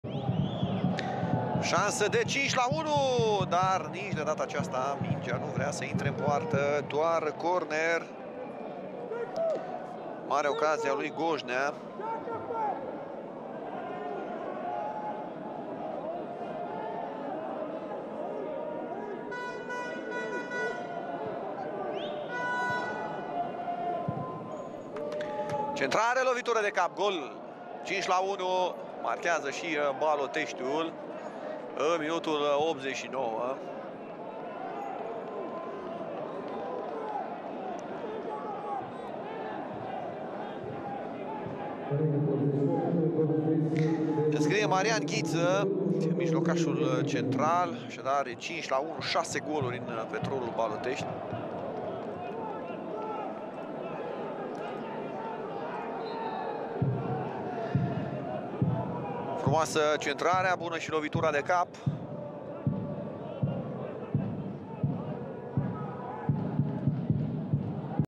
Petrolul - FC Balotesti 5-1, gol Ghita (88) Liga 2
Au marcat: Marinescu (33-pen, 84), Cazan (53) , Ghinga (61), Arnautu (77) / Ghita (88)

►Petrolul: Bolboaşă - Alb. Olaru, Fl. Plămadă, L. Cazan, Ţigănaşu - Ghinga, Ant. Cruceru - Tudor Şaim, Laur. Marinescu (cpt.), Rob. Grecu - Cl. Herea
Rezerve: Isvoranu - Antoche, N. Popescu, Ţicu, Ştefănescu, Arnăutu, M. Chindriş
Antrenor: Mugurel Cornăţeanu

►Baloteşti: Fulga - M. Neagu, Marciuc (cpt.), Flămânzeanu, G. Mihai - M. Ghiţă, C. Predescu, Ov. Marin, Gojnea - Vas. Mihai, Al. Stoica
Rezerve: Iordan - F. Paraschiv, Fl. Niculae, Clătinici, R. Mihai, N. Filip